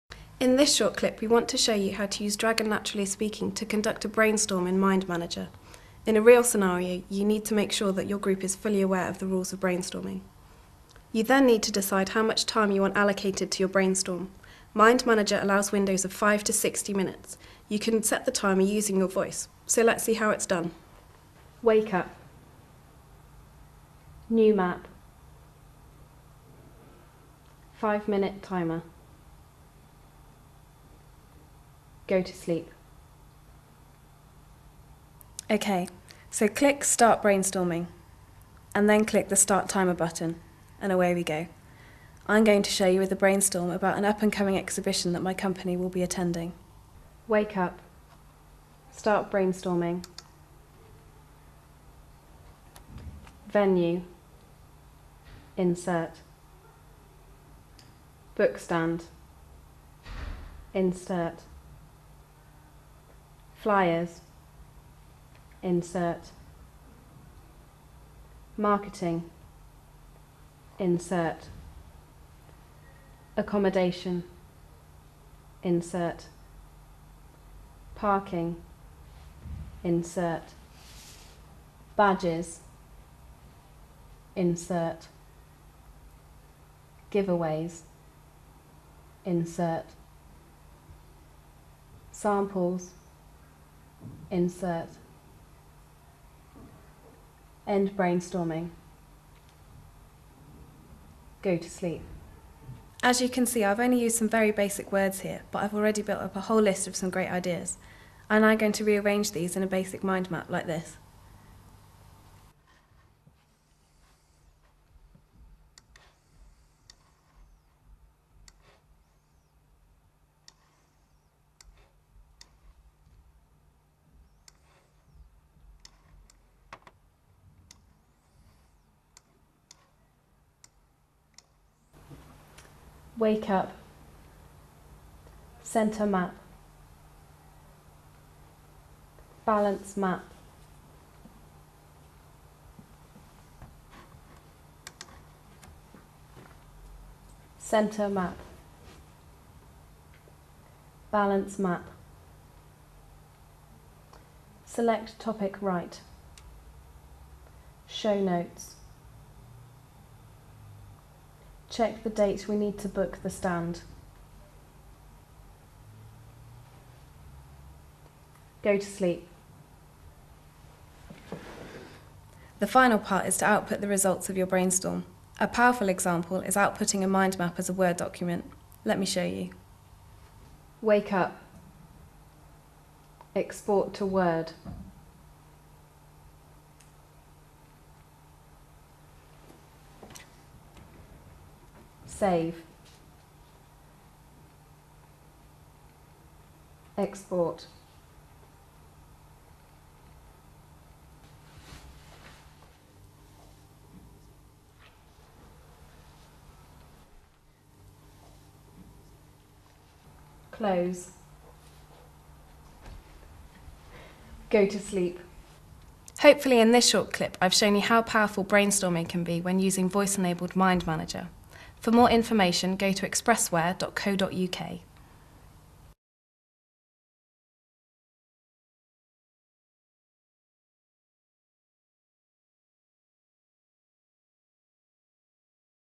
Brainstorming with 'voice-enabled' MindManager
Brainstorming by voice!  This video tutorial shows how you can produce a brainstorm just using your voice.